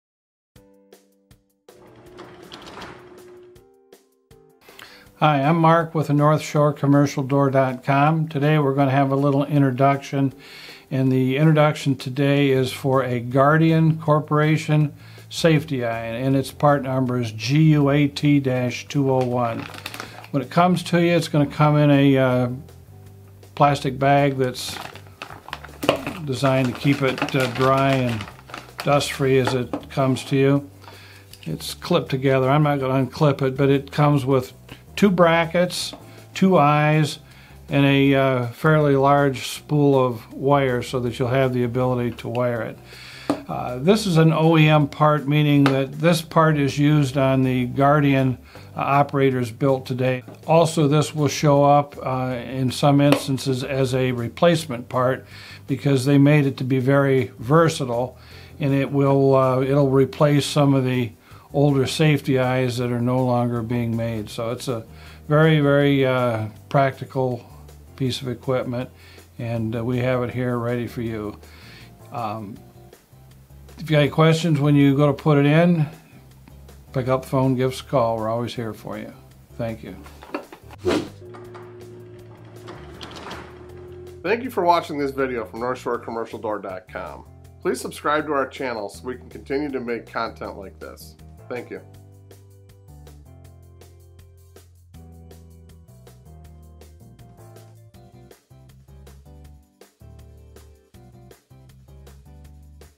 Guardian GUAT-201 Garage Door Opener Safety Beams Napoleon Lynx Proline
Guardian has updated their design for their safety eyes, and they are new and improved! These new Photo Beam Safety Sensors (former Napoleon/ Lynx Part PL-SW-PHOTO) come with everything assembled, and have a maximum range of 60 feet. In the package the wires, brackets, and sensors are already connected.
The safety eyes are compatible with:

Guardian:
• Core Series Model 315 ½ HP
• Guardian Core Series Model 415, ½ HP AC
• Guardian Core series Model 425, ½ HP AC
• Guardian Pro Series Model 615, ½ HPe DC
• Guardian Pro Series Model 628FCA, ¾ HPe DC, Music GDO

Napoleon/Lynx:
• Napoleon Lynx Proline 455 Series

Decko:
• 24000 ½ HP Chain Drive Garage Door Opener
• 24503 ¾ HP Garage Door Opener Belt Drive
• 24300 ¾ HP Chain Drive Garage Door Opener
• 24913
• 24914

Xtreme:
• Xtreme Garage 1/2 HP Electric Chain Drive Garage Door Opener 24000
• Xtreme Garage 1/2 HP Electric Chain Drive Garage Door Opener 425-1605
• Xtreme Garage ProSeries 3/4 HP Chain Drive Opener 425-1636
• Xtreme Garage ProSeries 3/4 HP Chain Drive Opener 24040
• Xtreme Garage ProSeries 3/4 HP Belt Drive Opener 425-1636
• Xtreme Garage ProSeries 3/4 HP Belt Drive Opener 425-1656
• Xtreme Garage ProSeries 3/4 HP Belt Drive Opener 24050

NorthShoreCommercialDoor.com Production